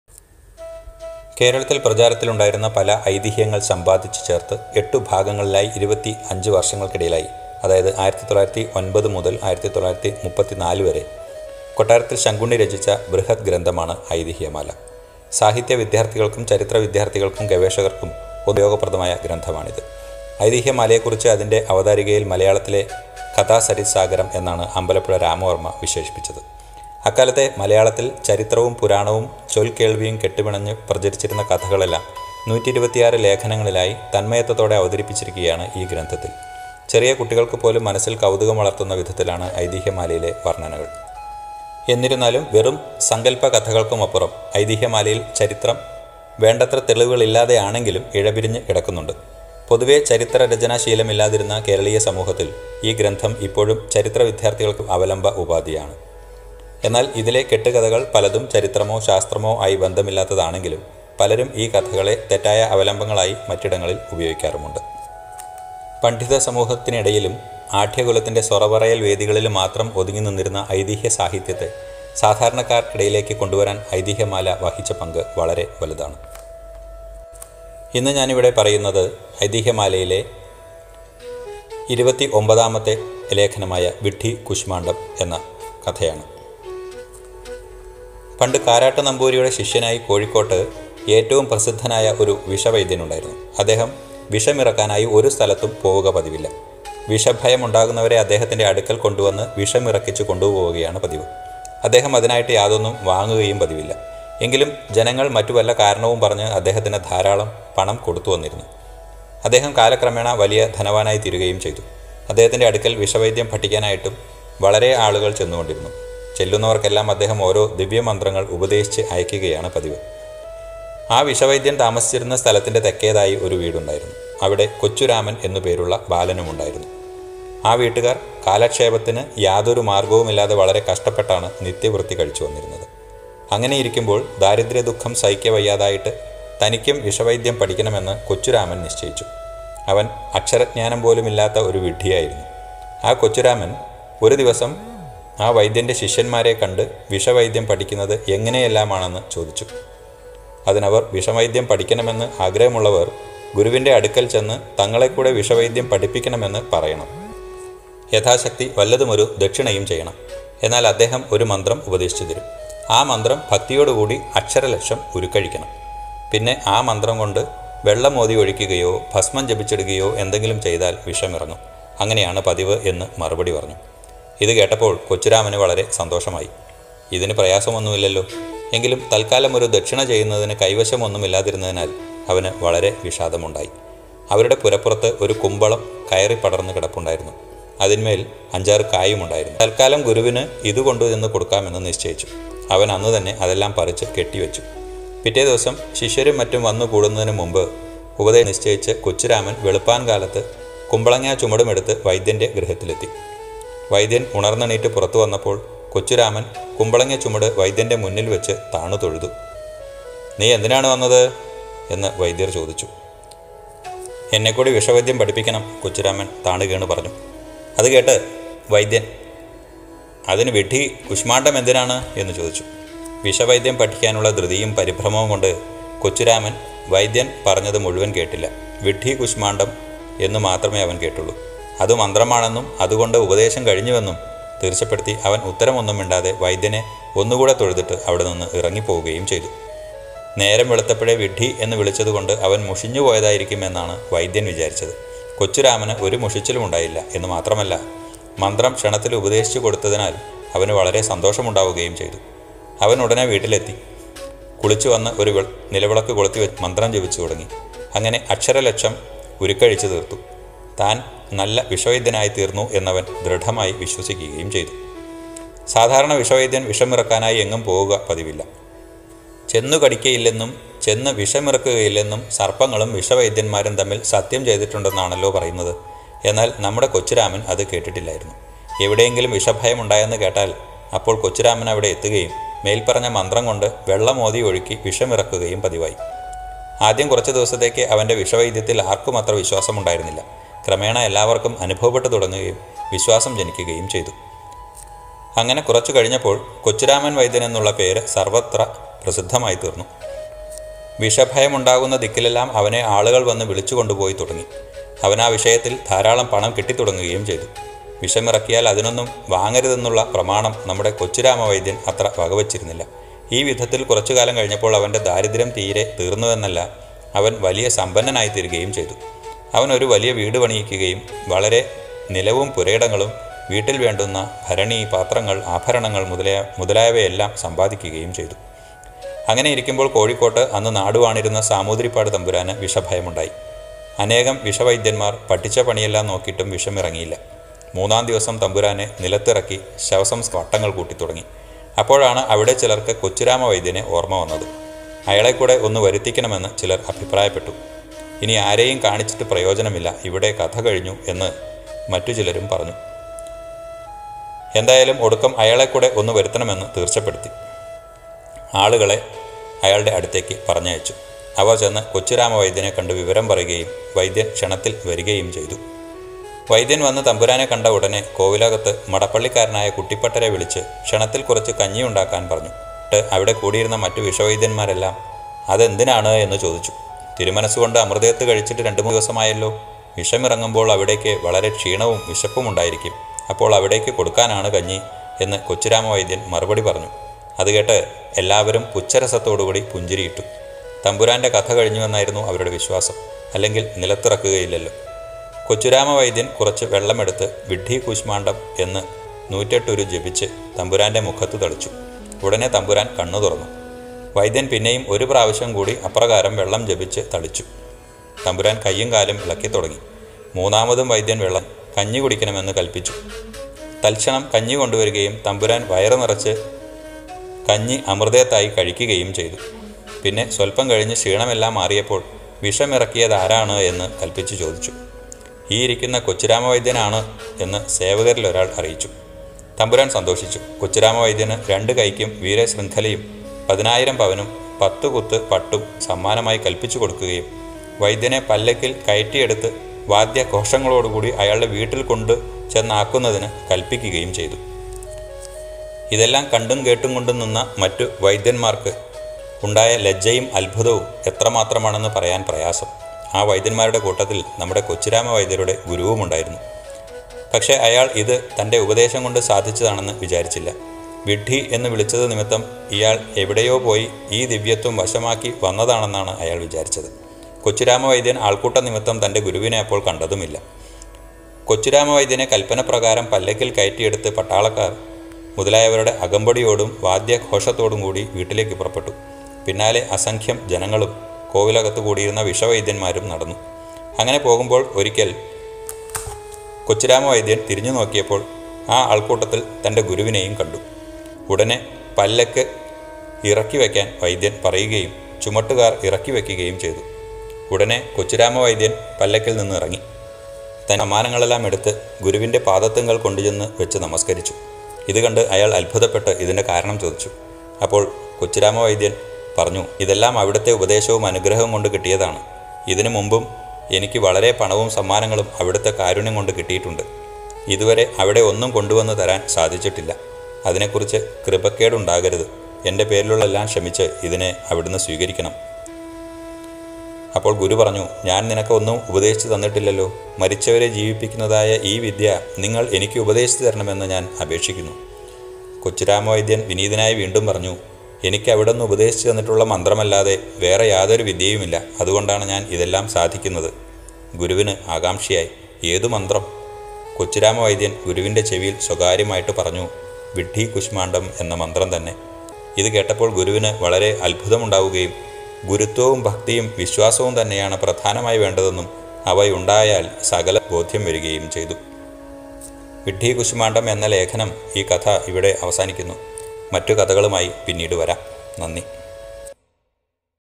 S014.02 | വിഡ്ഢി കൂശ്മാണ്ഡം | #29 ഐതിഹ്യമാല | കൊട്ടാരത്തിൽ ശങ്കുണ്ണി | Kottarathil Sankunni
കേരളത്തിൽ പ്രചാരത്തിലുണ്ടായിരുന്ന ഐതിഹ്യങ്ങളെല്ലാം സമ്പാദിച്ചു ചേർത്ത് എട്ടുഭാഗങ്ങളിലായി 25 വർഷങ്ങൾക്കിടയിലായി (1909 മുതൽ 1934 വരെ) കൊട്ടാരത്തിൽ ശങ്കുണ്ണി രചിച്ച ബൃഹദ്ഗ്രന്ഥമാണ് ഐതിഹ്യമാല. !

അക്കാലത്തെ മലയാളത്തിൽ ചരിത്രവും പുരാണവും ചൊൽക്കേൾവിയും കെട്ടുപിണഞ്ഞു പ്രചരിച്ചിരുന്ന കഥകളെല്ലാം 126 ലേഖനങ്ങളിലായി തന്മയത്വത്തോടെ അവതരിപ്പിച്ചിരിക്കുകയാണ് ഈ ഗ്രന്ഥത്തിൽ...

അതിലെ കഥകൾ/ലേഖനങ്ങൾ ഓരോന്നായി അവതരിപ്പിക്കാനുള്ള ശ്രമത്തിന്റെ തുടക്കമാണ് ഈ വീഡിയോ.

കടപ്പാട്: വിക്കിപീഡിയ